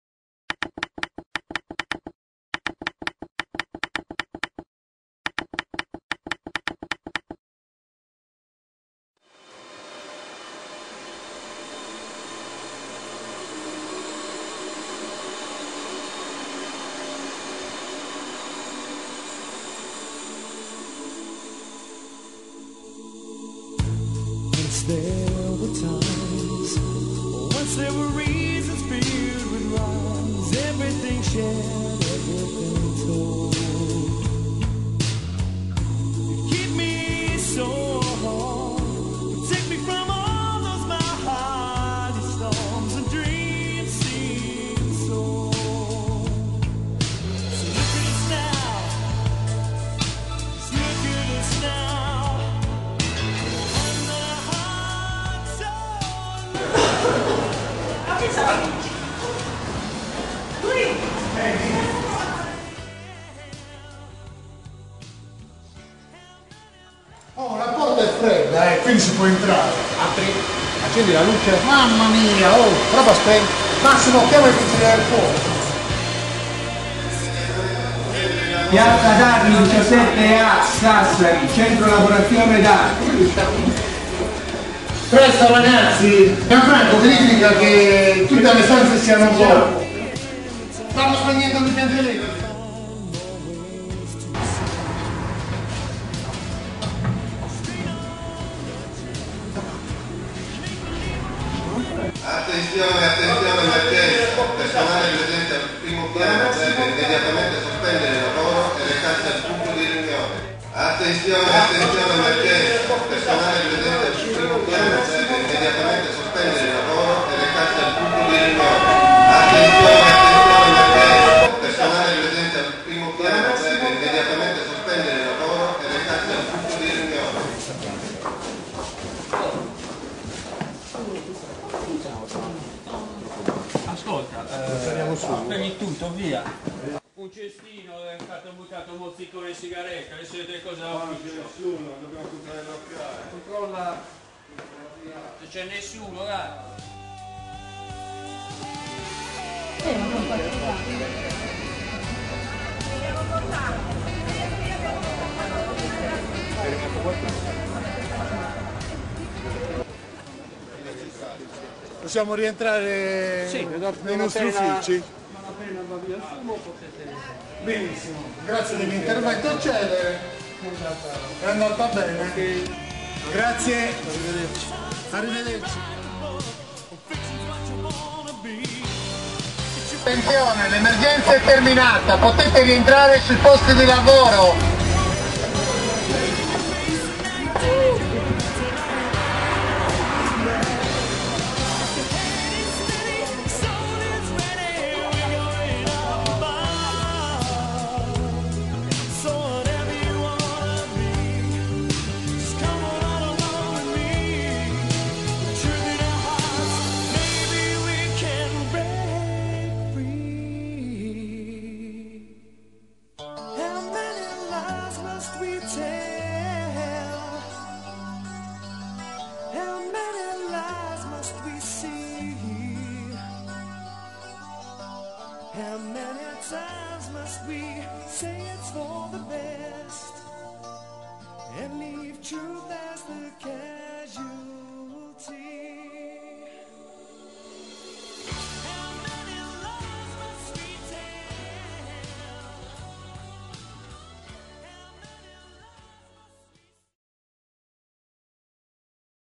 Simulazione antincendio CED Università degli Studi di Sassari
Simulazione antincendio effettuata nelle strutture del CED dell'Università degli Studi di Sassari